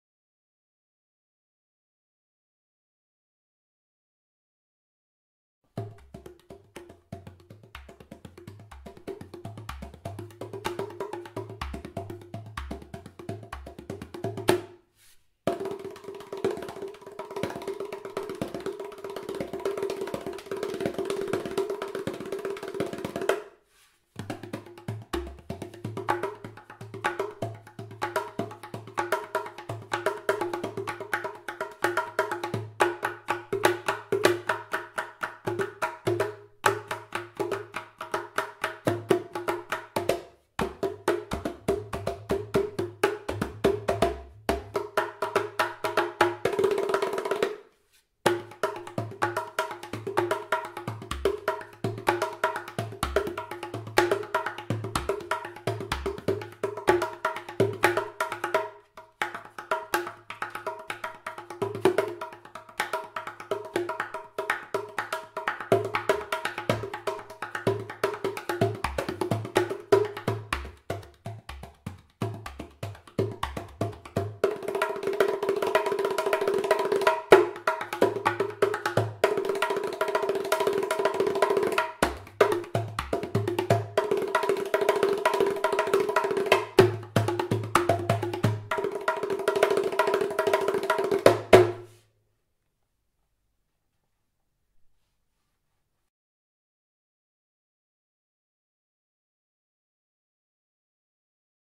Pejman Hadadi shows off his skills with the Majid Drums "Ta-Udu"
Pejman Hadadi shows his skills with the Ta Udu.